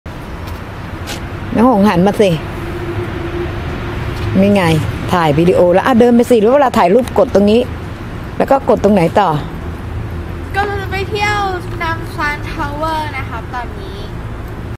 17-10-10 - heading to Namsan tower.MP4
Honggii and mama in seoul trip !!

หลังจาเก็บของแล้ว พร้อมลุย Namsan tower